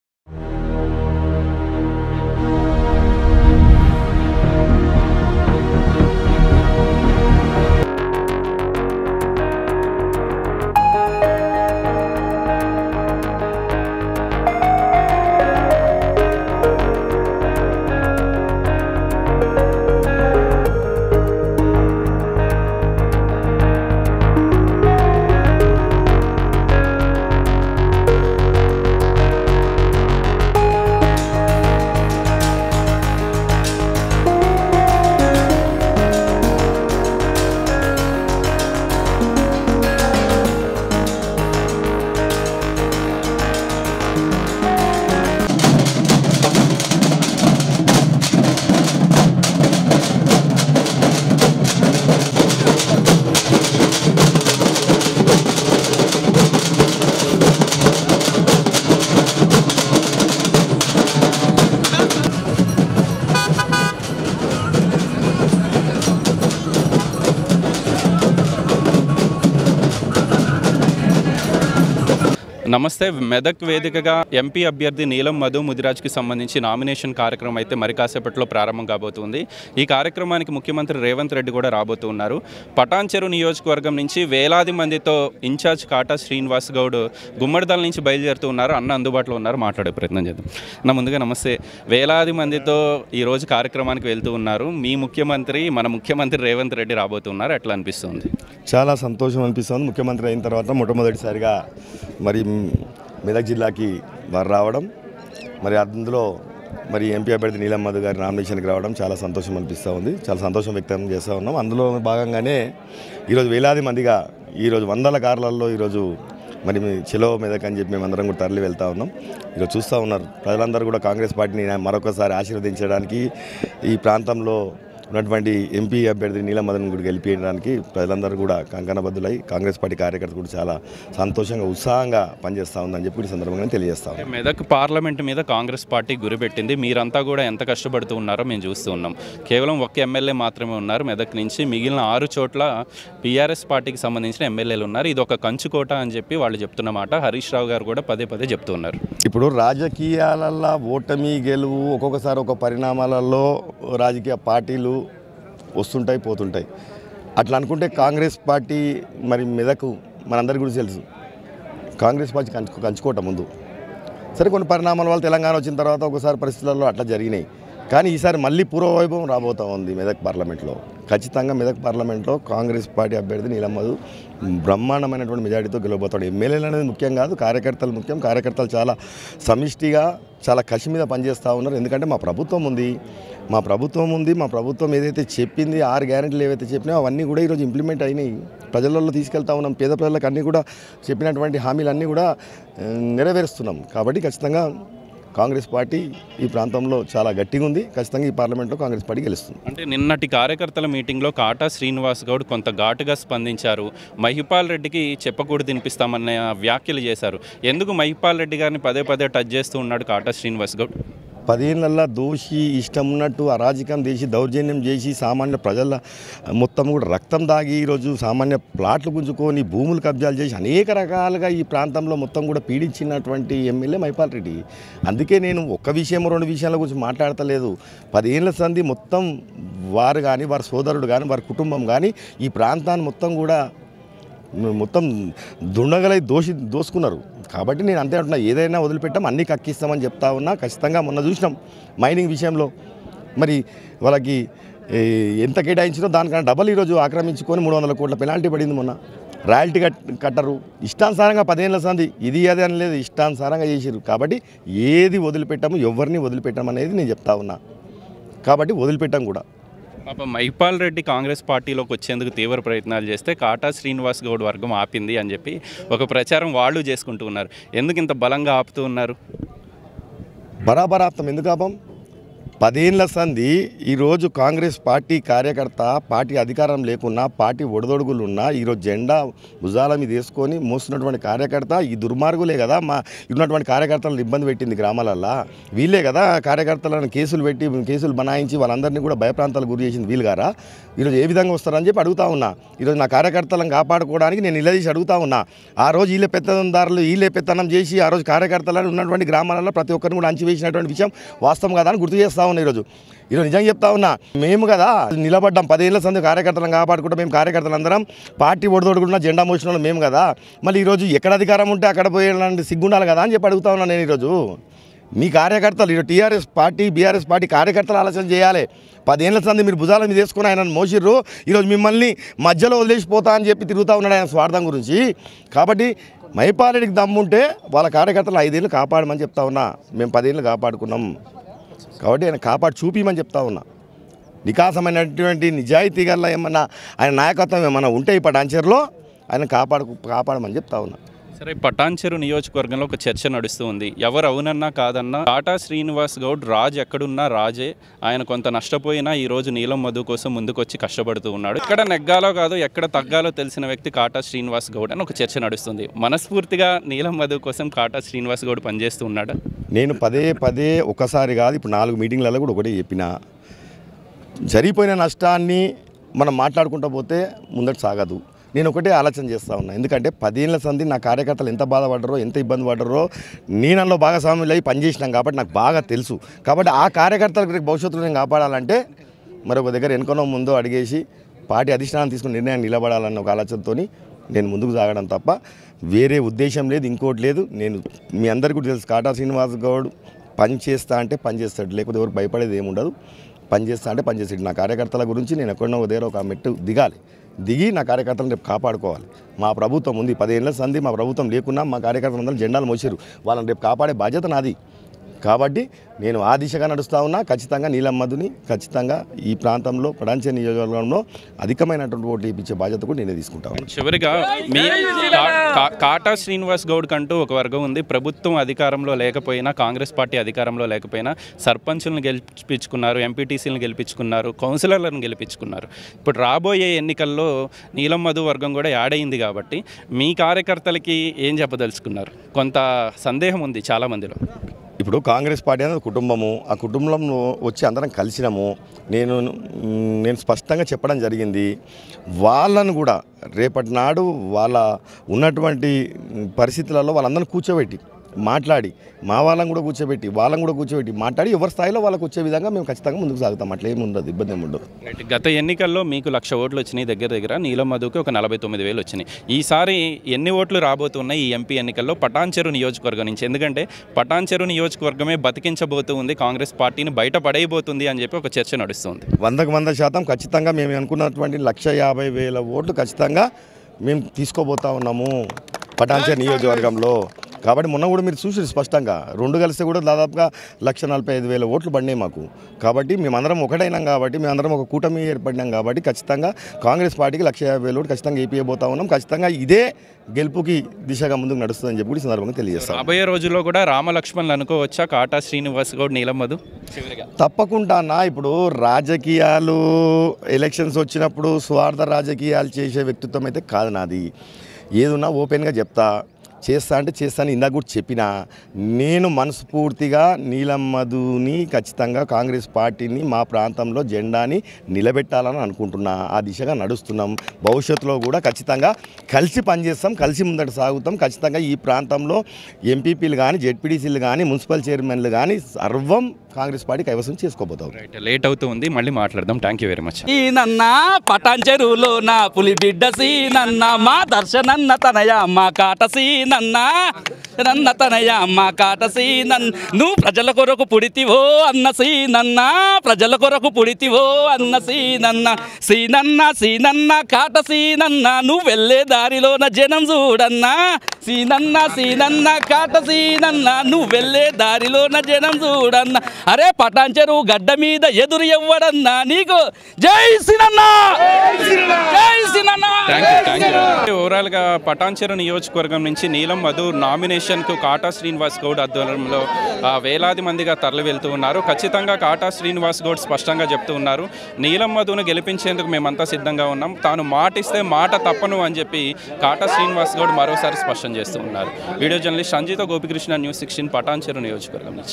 Kata Srinivas Goud : మాట ఇస్తే తప్పే చరిత్ర నాది కాదు | నీలం మధు గెలుపు తధ్యం | గూడెం గుండెలు అదరాలి
Kata Srinivas Goud : మాట ఇస్తే తప్పే చరిత్ర నాది కాదు | నీలం మధు గెలుపు తధ్యం | గూడెం గుండెలు అదిరేలా చేస్తా | NEWS16 Telugu

/ news16telugu​.  .